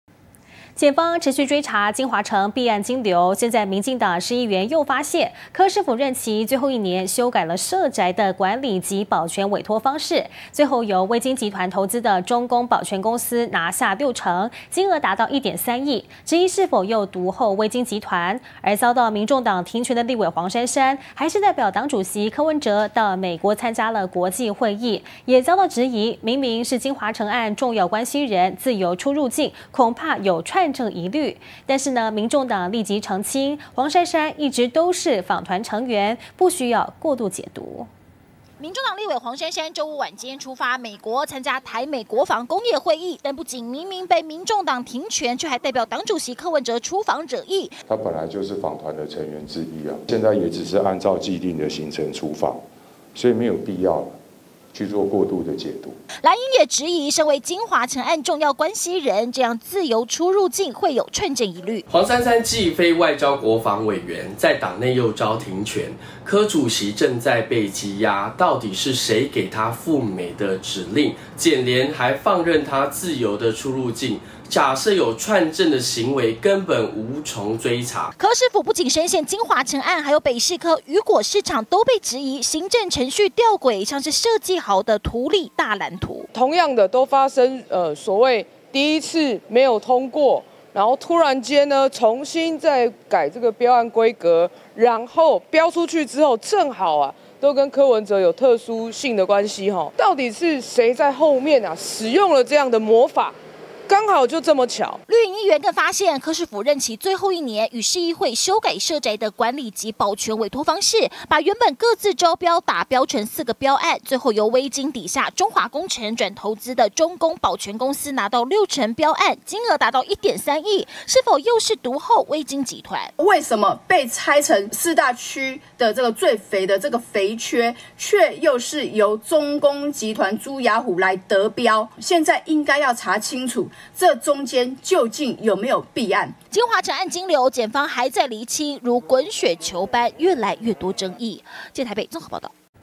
黃珊珊遭停權仍代柯赴美 藍營：恐有串證疑慮
檢方持續追查京華城弊案金流，現在民進黨市議員 又發現，柯市府任期最後一年，修改「社宅」的管理及保全委託方式，最後由威京集團投資的「中工保全公司」拿下6成、金額達到1.3億，質疑是否又獨厚 威京集團？而遭民眾黨停權的立委「黃珊珊」，還是代表 黨主席「柯文哲」到美國參加國際會議，也被質疑，明明是京華城案 重要關係人，自由出入境，恐怕有串證疑慮，但民眾黨立刻澄清，黃珊珊一直都是訪團成員、無須過度解讀。